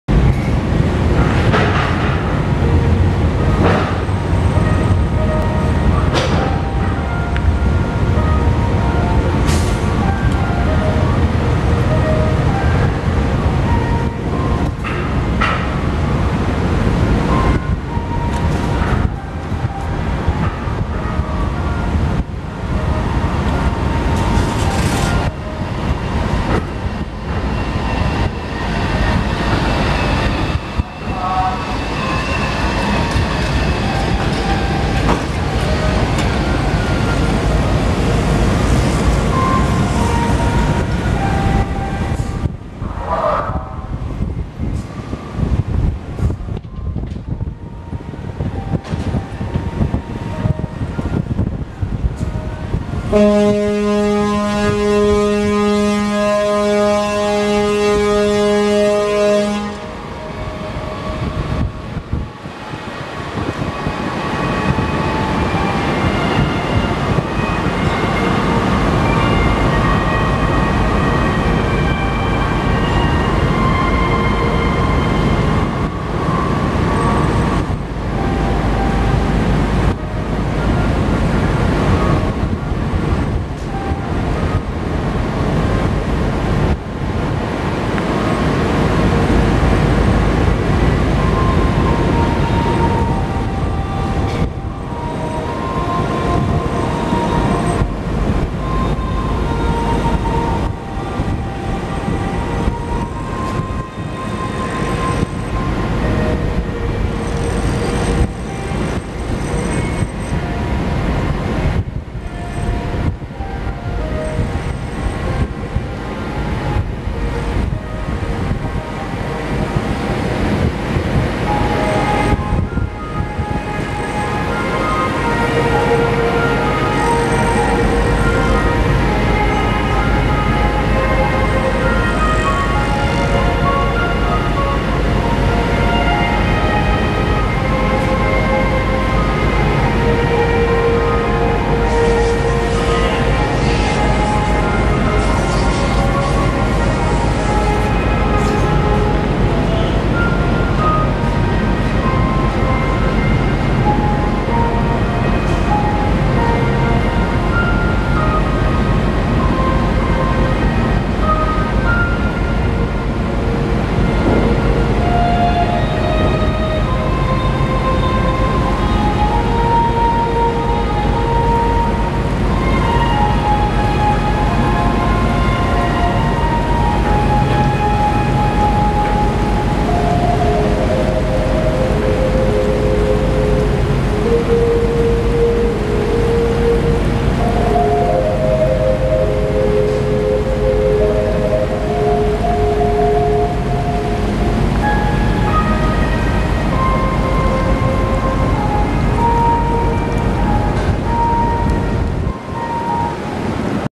底土港を出航する橘丸の船上から
八丈島にある底土港岸壁を離れる橘丸の船上から映しました。
２０１５年７月３日　１０：２１頃

この動画が使われているサイトは「東京都の風景」の「底土港の橘丸」です。